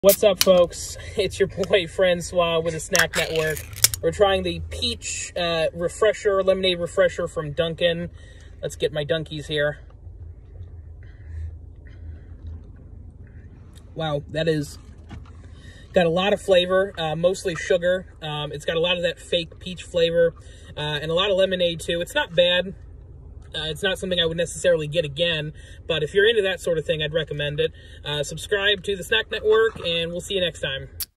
Dunkin' Peach Lemonade Refresher Taste Test With Ben!
Dunlin has new Lemonade refreshers. The entire team is here to try all 3 flavors. Ben is here as well to taste the Peach Lemonade refresher. Is he as big of a fan as Dave?